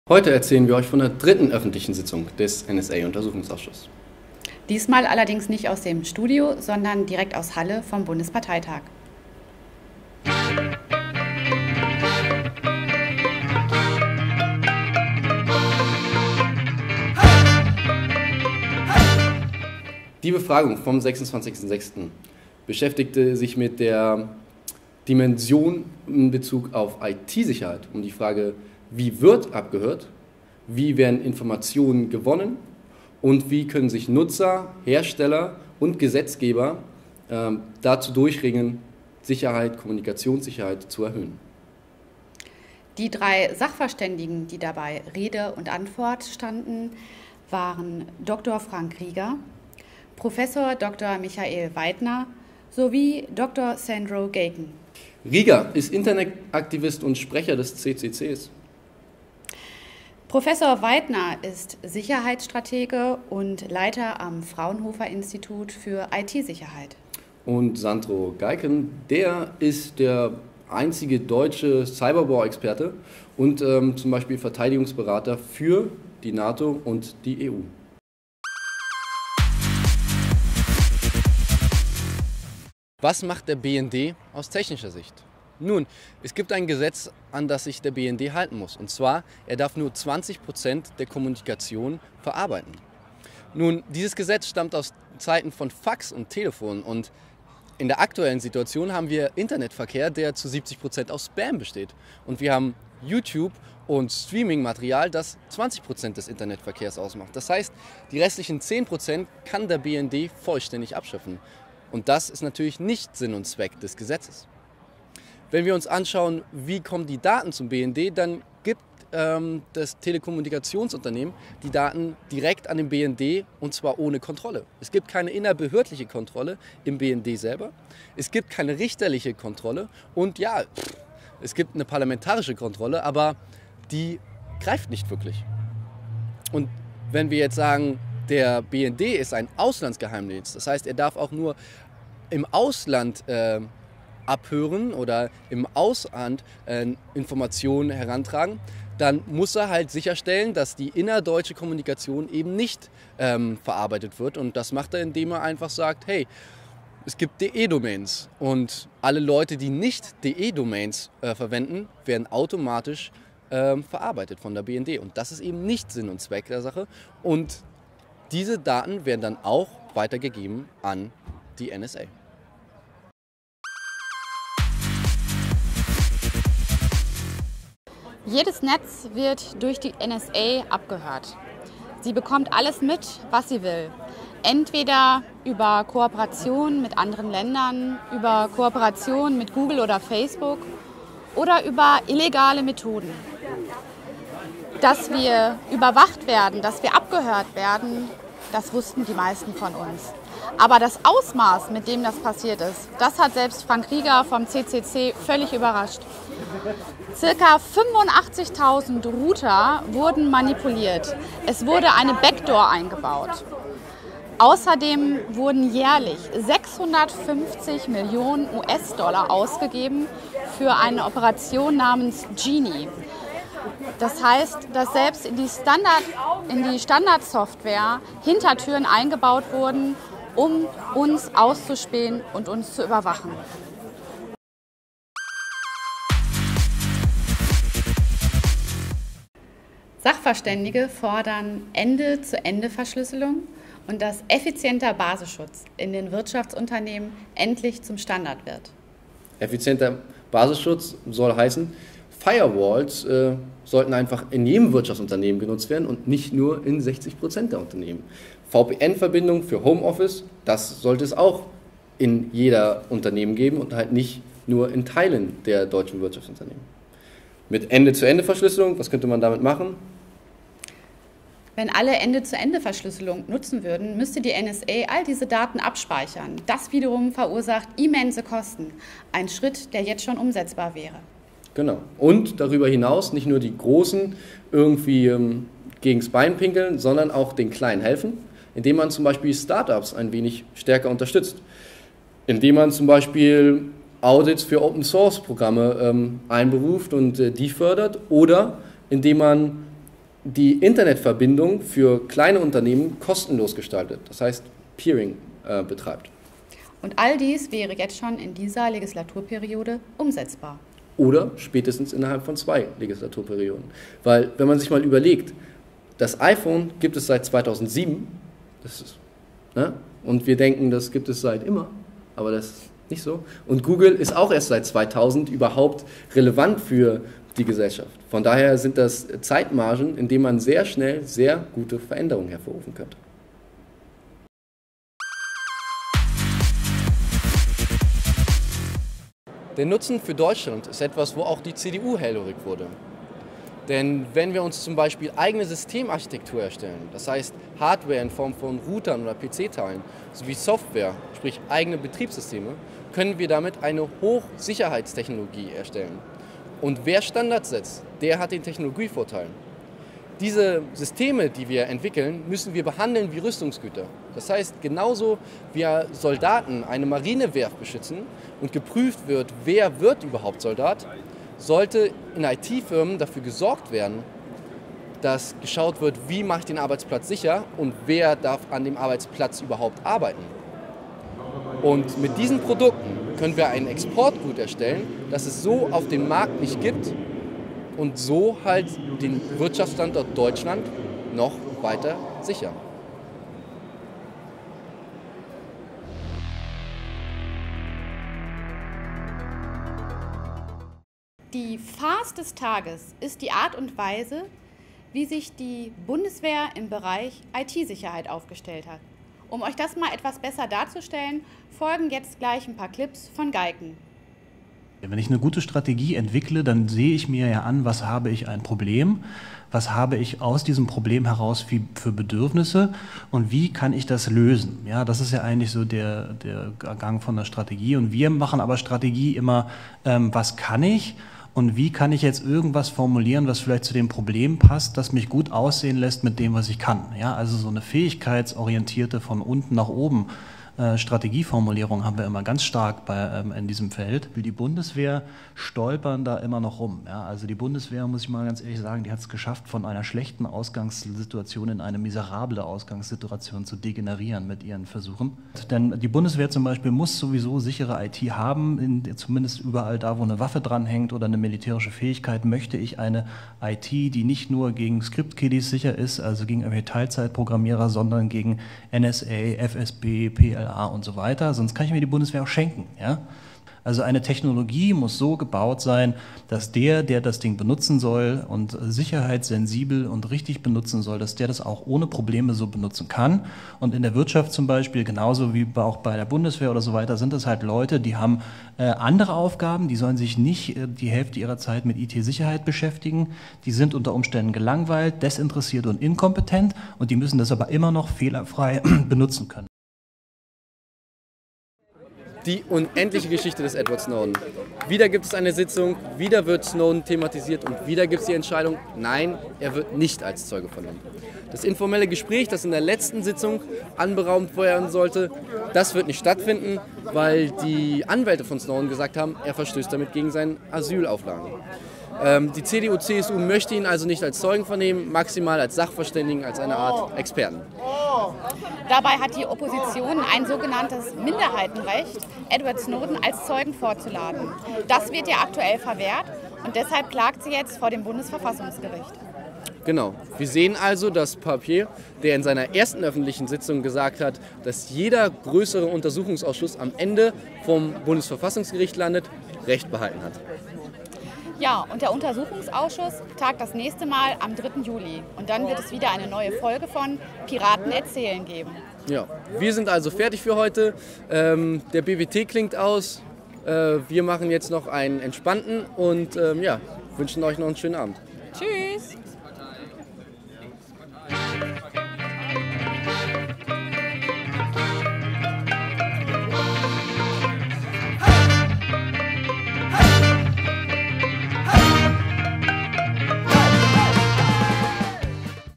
NSA-Ausschuss: Exportgut Datenschutz
Parlamentsfernsehen: CC-BY Bundestag

Heute erzählen wir euch von der 3. öffentlichen Sitzung des NSA-Untersuchungsauschuss.
Diesmal jedoch nicht aus unserem Studio in Berlin, sondern  direkt vom Bundesparteitag in Halle.

Die Sitzung vom 26.06. beschäftigte sich mit der Frage welche technische Dimension die weltweite Spionage erreicht hat und welche Gegenmaßnahmen man einleiten kann um sich dagegen zu schützen.